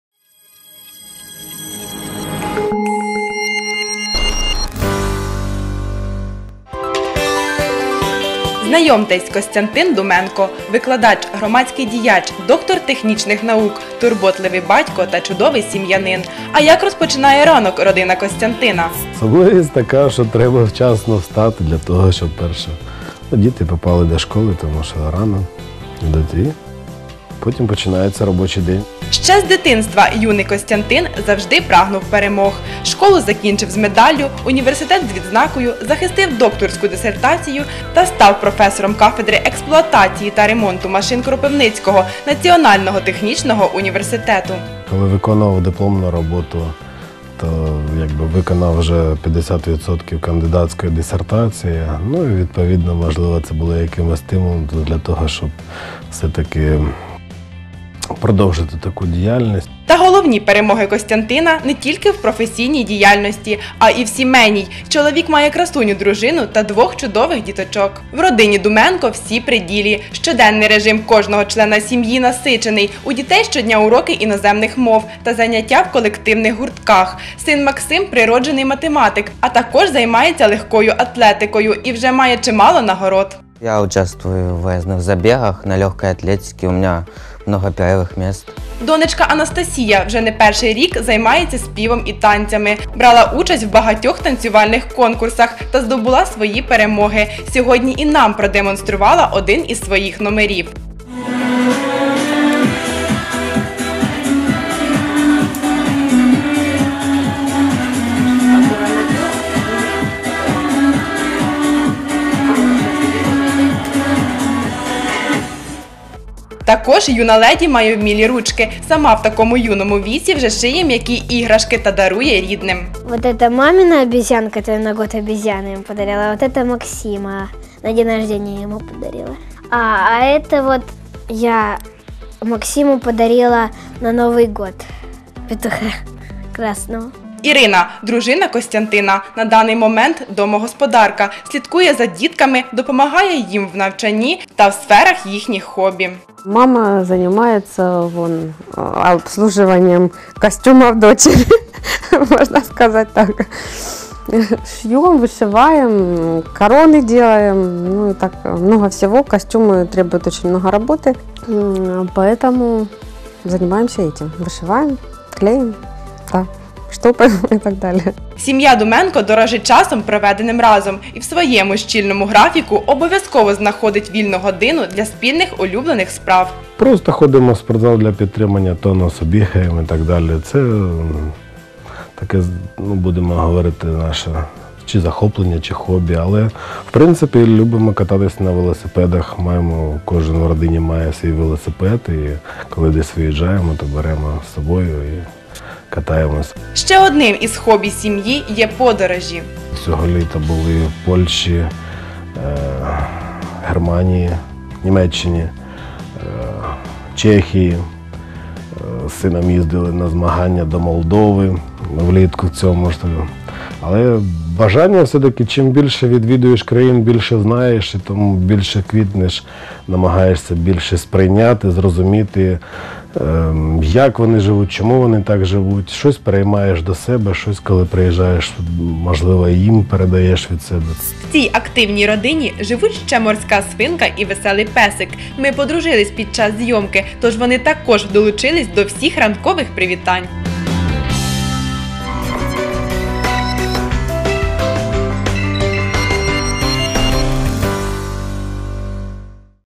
Ранок чудових людей
Герой рубрики - Костянтин Думенко.